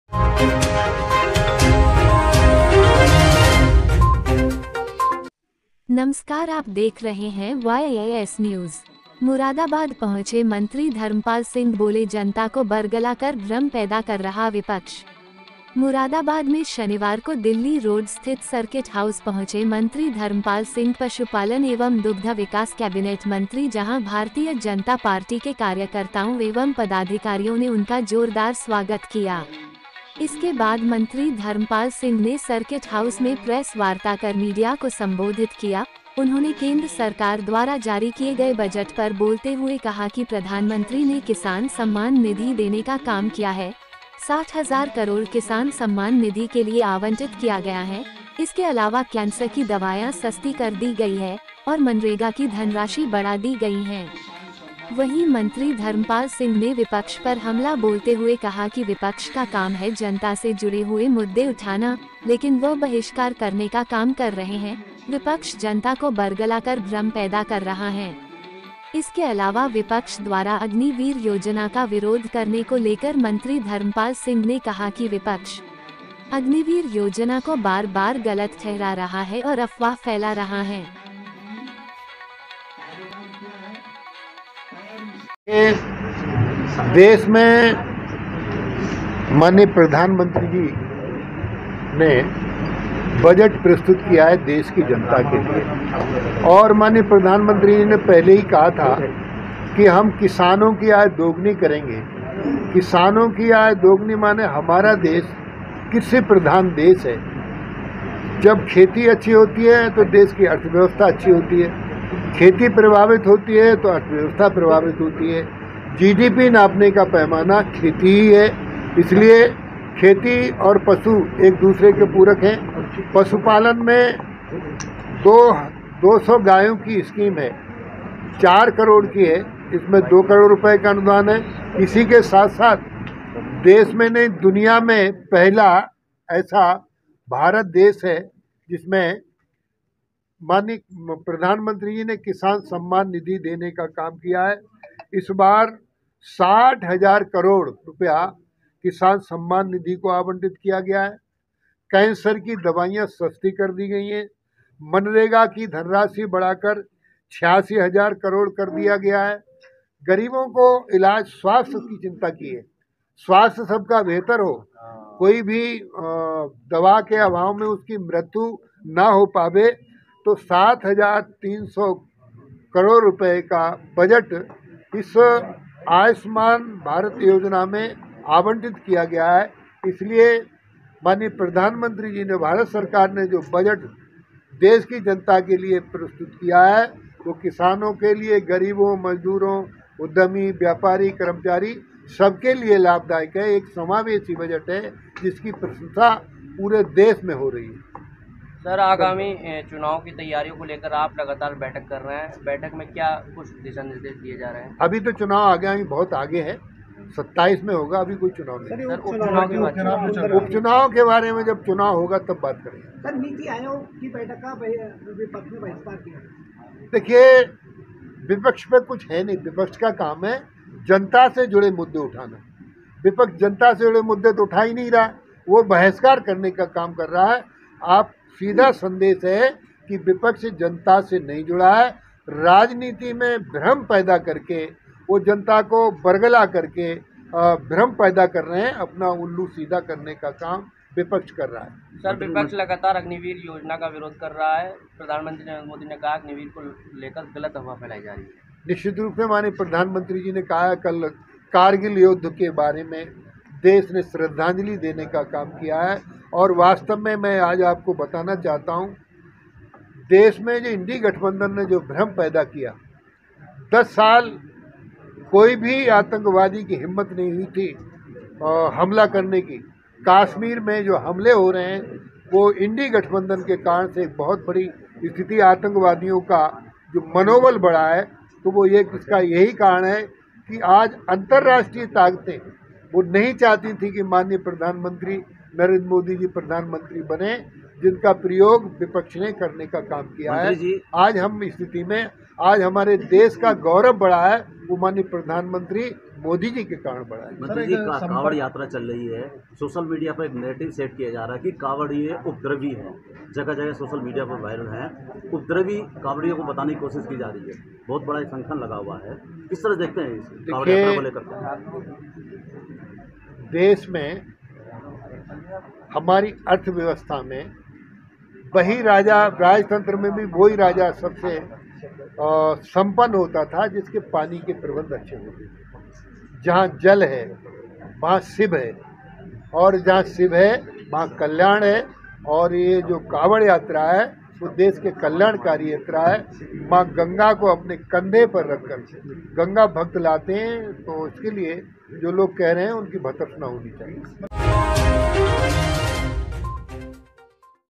मुरादाबाद पहुंचे मंत्री धर्मपाल सिंह बोले जनता को बरगला कर-भ्रम पैदा कर रहा विपक्ष।
मुरादाबाद में शनिवार को दिल्ली रोड स्थित सर्किट हाउस पहुंचे- मंत्री धर्मपाल सिंह पशुपालन एवं दुग्ध विकास कैबिनेट मंत्री जहां भारतीय जनता पार्टी के कार्यकर्ताओं एवं पदाधिकारियों ने उनका जोरदार स्वागत किया, इसके बाद मंत्री धर्मपाल सिंह ने सर्किट हाउस में प्रेस वार्ता कर मीडिया को संबोधित किया, उन्होंने केंद्र सरकार द्वारा जारी किए गए बजट पर बोलते हुए कहा कि प्रधानमंत्री ने किसान सम्मान निधि देने का काम किया है, 60 हज़ार करोड़ किसान सम्मान निधि के लिए आवंटित किया गया हैं, इसके अलावा कैंसर की दवाइयां सस्ती कर दी गई है, और मनरेगा की धनराशि बढ़ा दी गई हैं, वहीं मंत्री धर्मपाल सिंह ने विपक्ष पर हमला बोलते हुए कहा कि विपक्ष का काम है जनता से जुड़े हुए मुद्दे उठाना, लेकिन वह बहिष्कार करने का काम कर रहे हैं, विपक्ष जनता को बरगला कर भ्रम पैदा कर रहा हैं, इसके अलावा विपक्ष द्वारा अग्नि वीर योजना का विरोध करने को लेकर मंत्री धर्मपाल सिंह ने कहा कि विपक्ष अग्निवीर योजना को बार-बार गलत ठहरा रहा है और अफवाह फ़ैला रहा हैं।